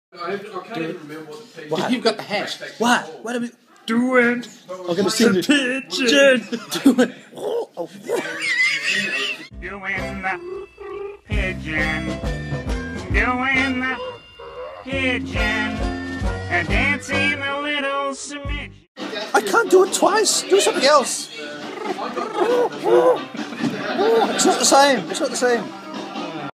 Doin' the Pigeon
Adrian dancing a litte smidgeon of the kind of ballet that sweeps me away.